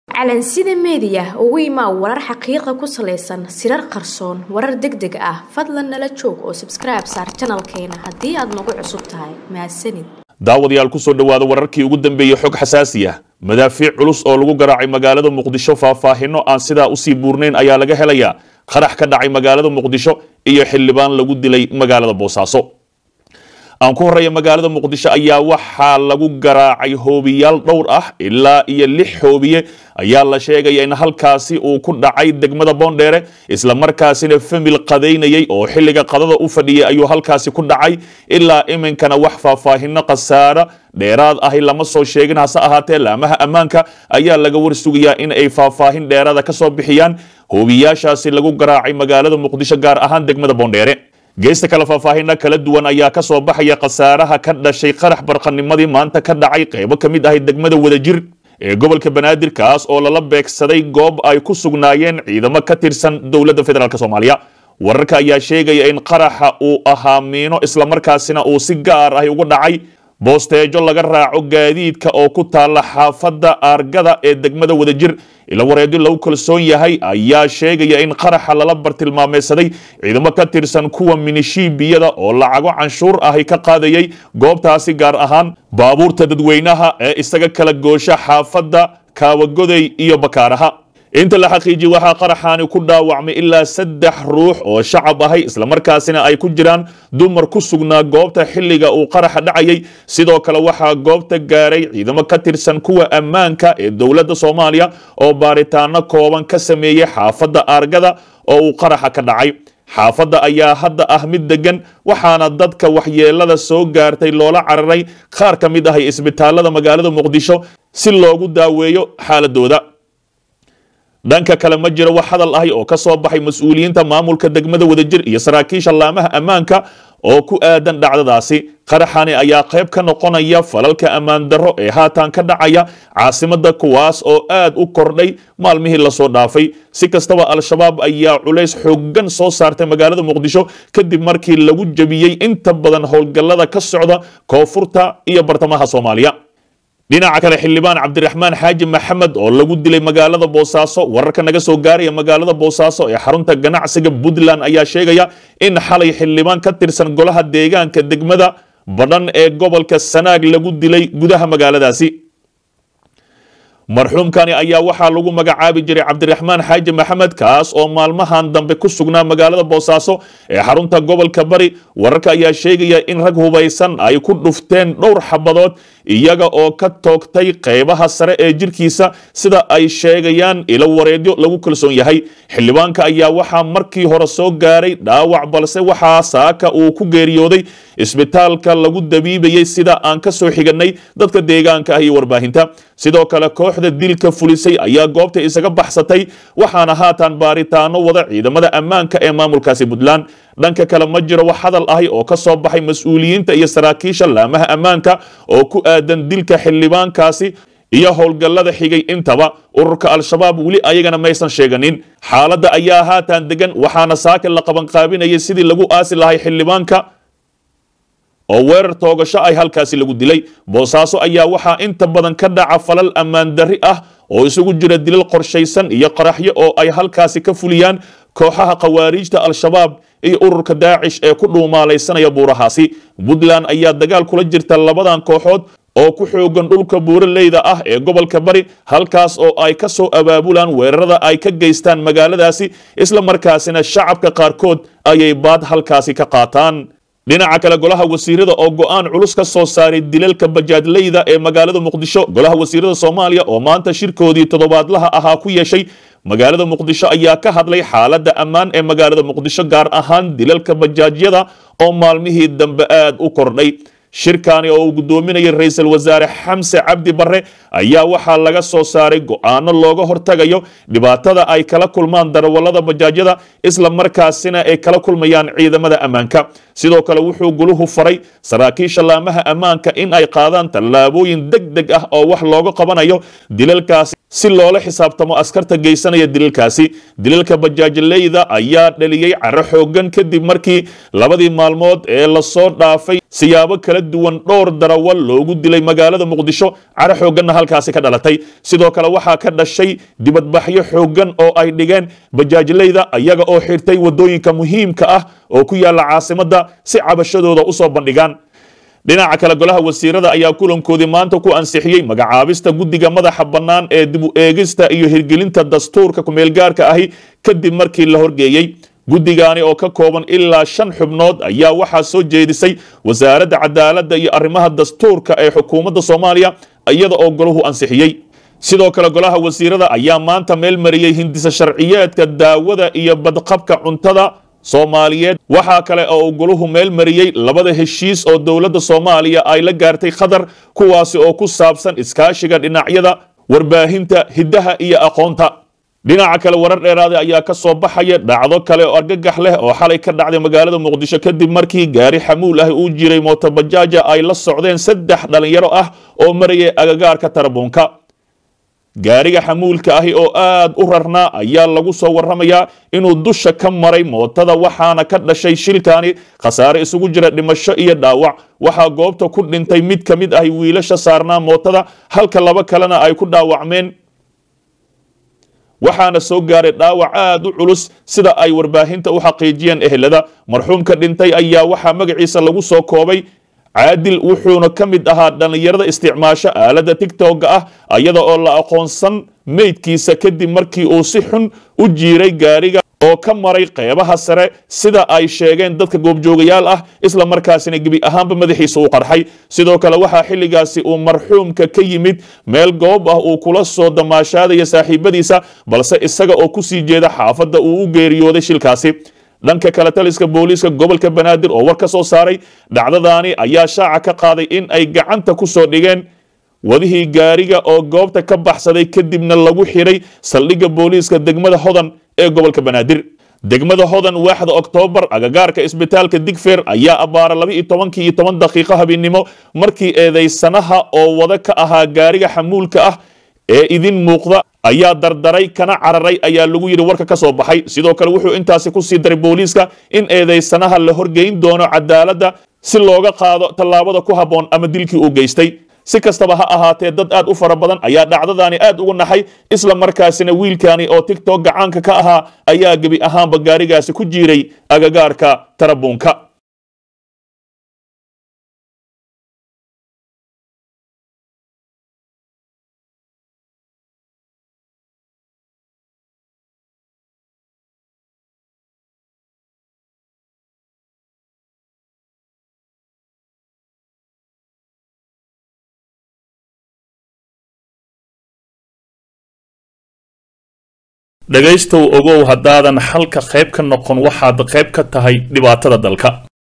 DEG DEG Xildhibaan la dilay Qarax ka dhacay Muqdisho & Hoobiyayaal lagu xasuuqay shacab is casila Fa
DEG DEG Xildhibaan la dilay Qarax ka dhacay Muqdisho & Hoobiyayaal lagu xasuuqay shacab is casila Fadlan ,madaxda dalka, xog culus wararkii ugu dambeyay